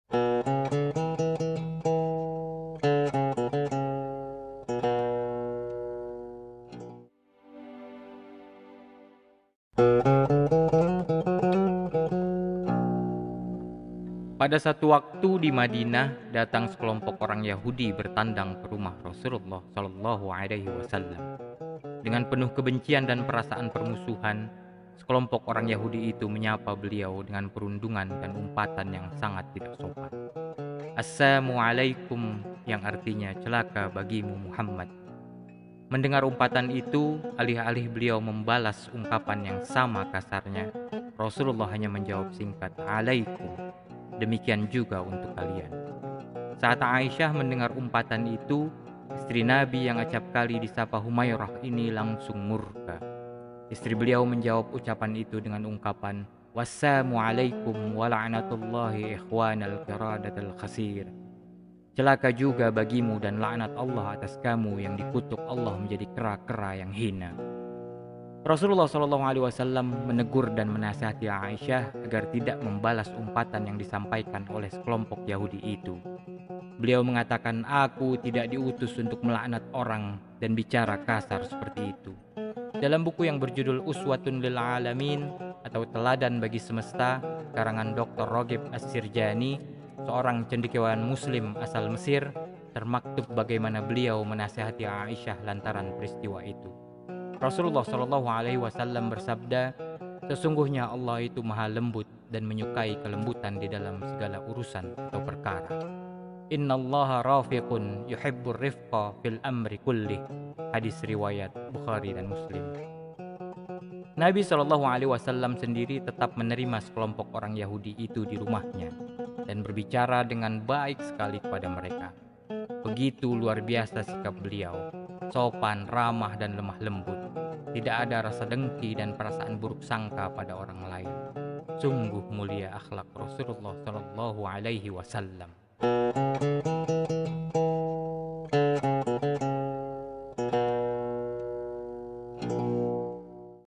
Cara Rasulullah SAW Menghadapi Bullying | Ust. Achmad Rifki I Tauhid Cinta Channel
Pada satu waktu, di Madinah, datang sekelompok orang Yahudi bertandang ke rumah Rasulullah SAW. Dengan penuh kebencian dan perasaan permusuhan, sekelompok orang Yahudi itu menyapa beliau dengan perundungan (bullying) dan umpatan yang sangat tidak sopan: “Al-saammu ‘alaykum” (celaka bagimu [Muhammad]).

Mendengar umpatan itu, alih-alih beliau membalas ungkapan yang sama kasarnya, Rasulullah hanya menjawab singkat: "‘alaykum" (demikian juga untuk kalian).

Saat Aisyah mendengar umpatan itu, istri Nabi yang acapkali disapa Humairah ini langsung murka. Istri beliau menjawab ucapan itu dengan ungkapan, “Was sammu ‘alayka wa la‘natullaahi ikhwaanal qiraadatal khasiir” (celaka juga bagimu dan laknat Allah atas kamu yang dikutuk Allah menjadi kera-kera yang hina).

Rasulullah SAW menegur dan menasihati ‘Aisyah agar tidak membalas umpatan yang disampaikan oleh sekolompok Yahudi itu. Beliau mengatakan: “Aku tidak diutus untuk melaknat orang dan bicara kasar seperti itu.”

Dalam buku yang berjudul “Uswatun lil ‘Aalamiin” (Teladan bagi Semesta) karangan Dr. Raghib As-Sirjani, seorang cendekiawan muslim asal Mesir, termaktub bagaimana beliau menasihati Aisyah lantaran peristiwa itu.

Rasulullah SAW bersabda, “Sesungguhnya Allah itu Maha Lembut dan menyukai kelembutan di dalam segala urusan/perkara. (Innallaaha Rafiiqun, yuhibbur rifqa fil amri kullih) [Hadis riwayat Bukhari dan Muslim].

Nabi SAW sendiri tetap menerima sekolompok orang Yahudi itu di rumahnya dan berbicara dengan baik sekali kepada mereka. Begitu luar biasa sikap beliau. Sopan, ramah, dan lemah lembut. Tidak ada rasa dengki dan perasaan buruk sangka pada orang lain.

Sungguh mulia akhlak Rasulullah SAW.

Yuk, sedekah dengan jari kita. Terima kasih atas apresiasinya. Saran dan kritik yang membangun dari teman-teman sangat kami harapkan dari setiap video kami.

Semoga Allah SWT senantiasa melimpahi kita keberkahan dalam hidup. Amin.

Salam Tauhid, Salam Cinta untuk Semesta.